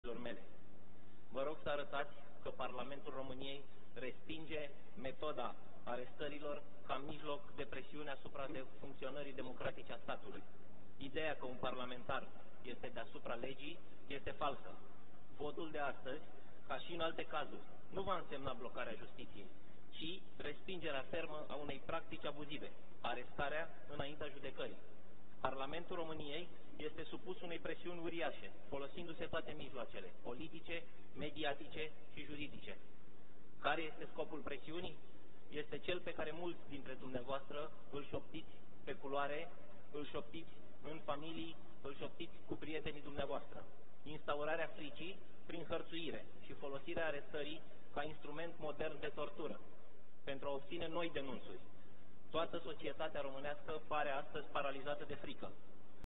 Ședința în plen a Camerei Deputaților României din 30 martie 2016
Sebastian Ghiță: Care este scopul presiunii? Cel pe care mulți îl șoptiți pe culoare, în familii, cu prietenii dumneavoastră - instaurarea fricii prin hărțuire și folosirea arestării ca instrument modern de tortură.
Titlul evenimentului: Ședința în plen a Camerei Deputaților României din 30 martie 2016